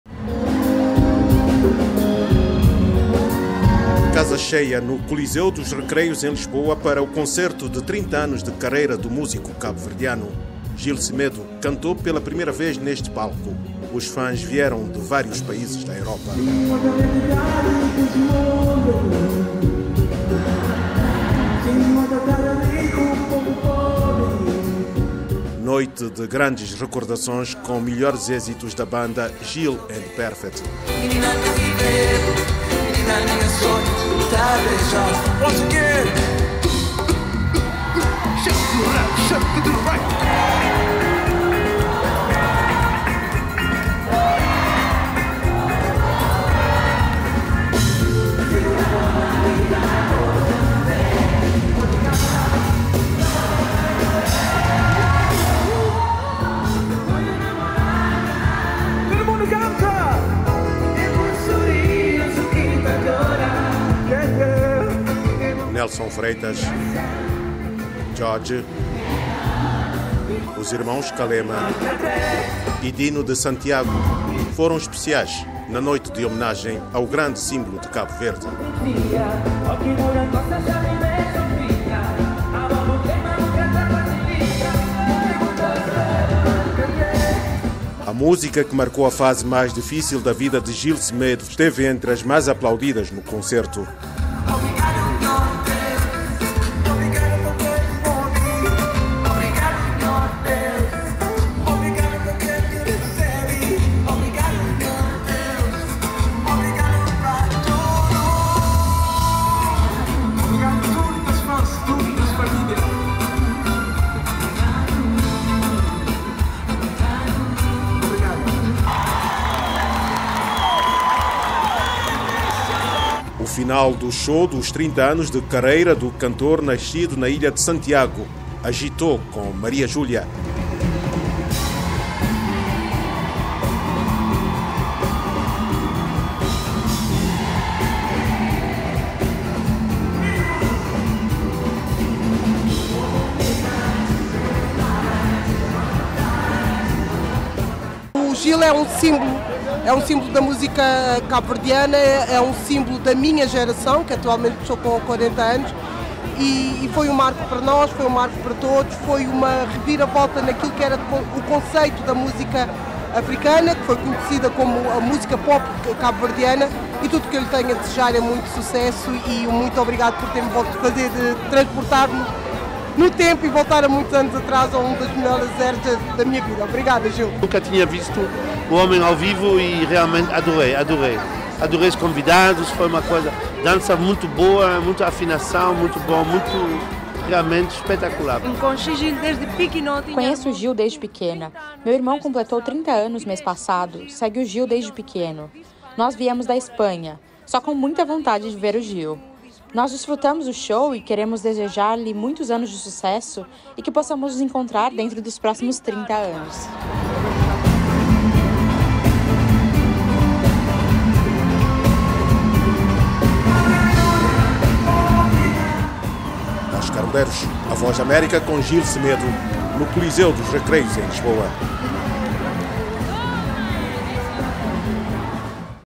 30 anos de Gil Semedo
O músico cabo-verdiano, Gil Semedo assinalou 30 anos de carreira com um concerto em Lisboa cheio de emoções. Artistas como Nelson Freitas  e os Calema também fizeram parte da celebração.
O  nosso correspondente Óscar Medeiros falou com alguns fãs do artista depois do show.